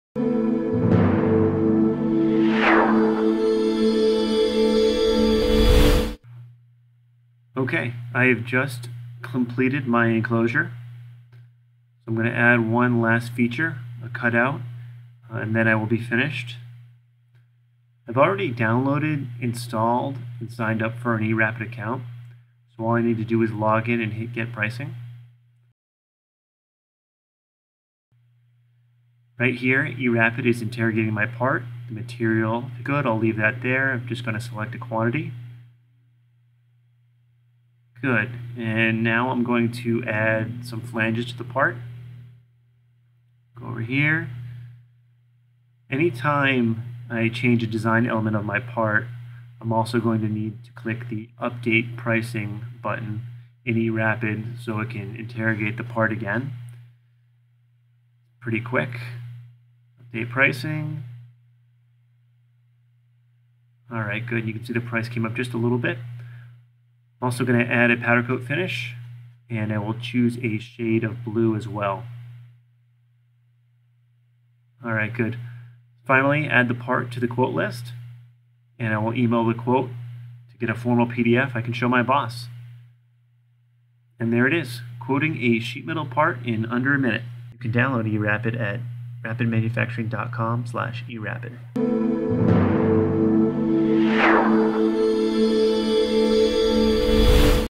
Instant Sheet Metal Part Quotes - eRAPID SOLIDWORKS Plugin
Quote a sheet metal part in less than 1 minute!

eRAPID is a FREE instant sheet metal part quoting & ordering add-in for SOLIDWORKS. Reduce time to market – Design your parts for manufacturing at the start of the process, not during the RFQ stage. From a simple bracket to a full sheet metal assembly, eRAPID will quote your sheet metal parts, INSTANTLY.